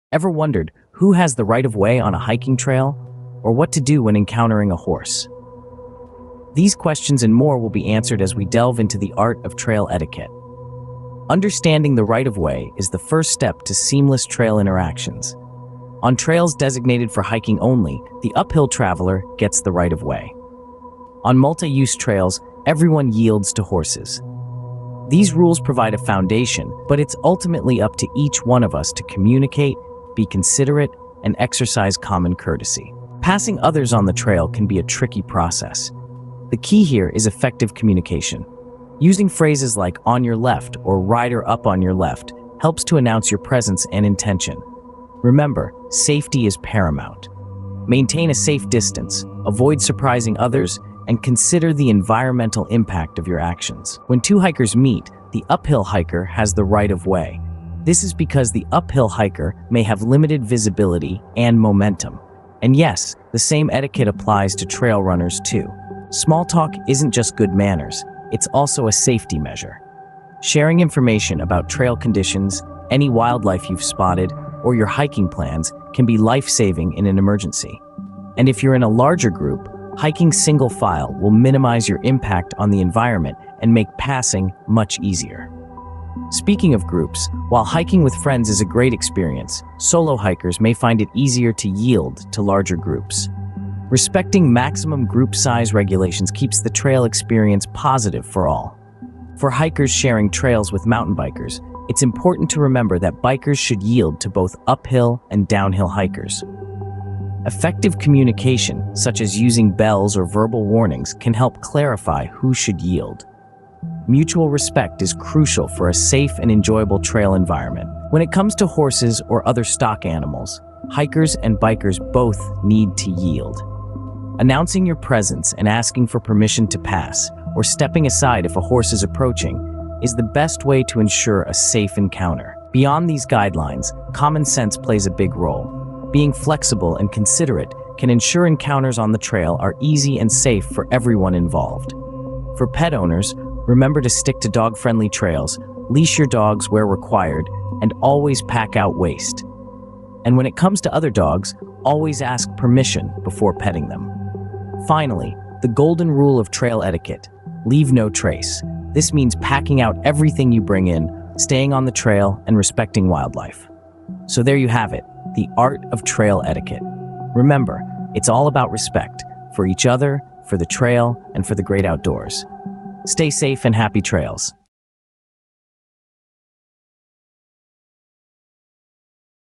Mastering Trail Etiquette: A Guide for Outdoor Enthusiasts
As the popularity of hiking and outdoor activities continues to soar, it becomes increasingly crucial for trail-goers to embrace and practice good trail etiquette. Proper trail etiquette ensures a positive and safe experience for everyone, while also preserving the integrity of trails and protecting wildlife. In this comprehensive guide, we delve into the fundamentals of trail etiquette, covering right-of-way basics, passing etiquette, small talk, group dynamics, and specific considerations for hikers, mountain bikers, and pet owners.